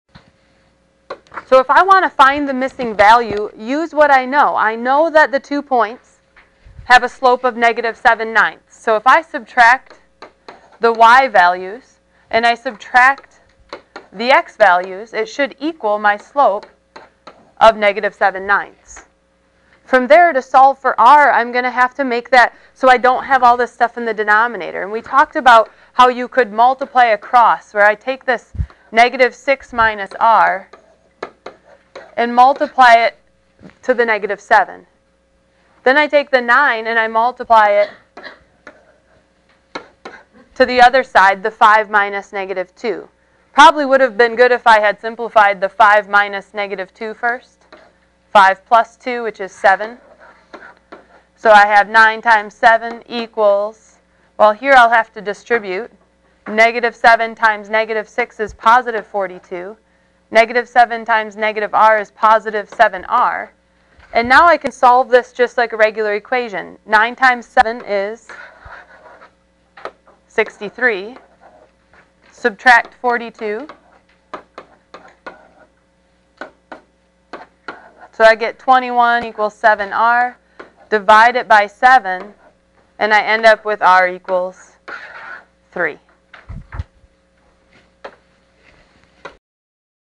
Find missing coordinate when know slope
Find the missing value in a coordinate when you know the slope